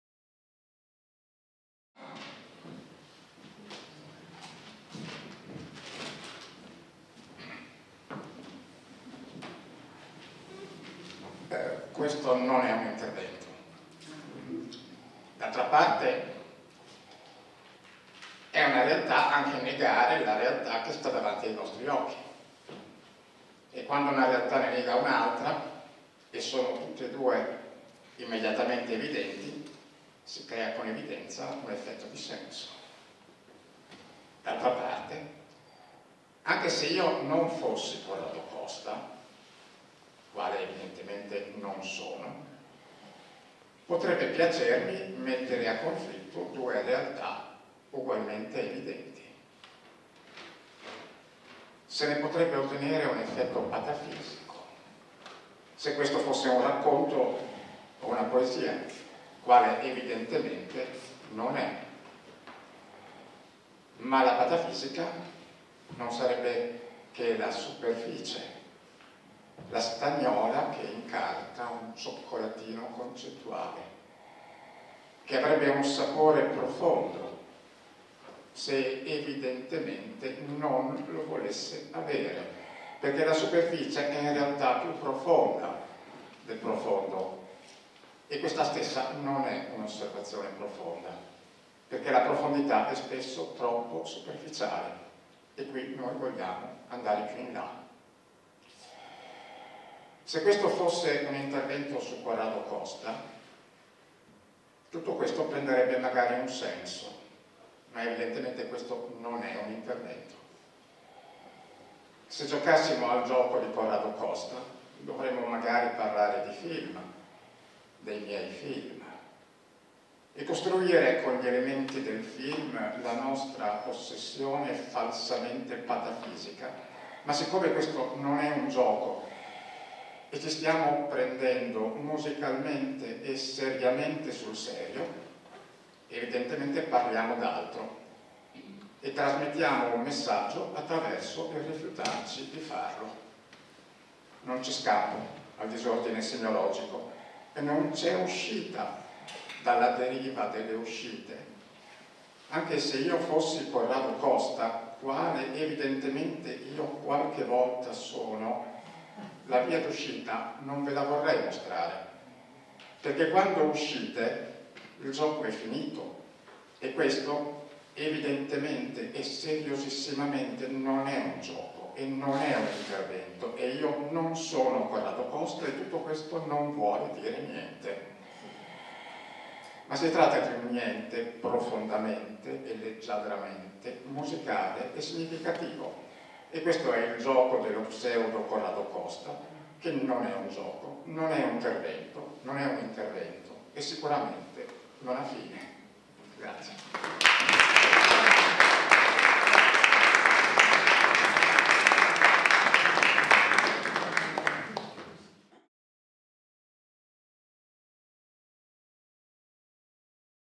Daniele Barbieri Questo non è un titolo
Bologna in Lettere - VIII edizione - Sabato 14 dicembre 2019 - "il_titolo_lo_mettiamo_dopo (una serata per Corrado Costa)" - Daniele Barbieri "Questo non è un titolo"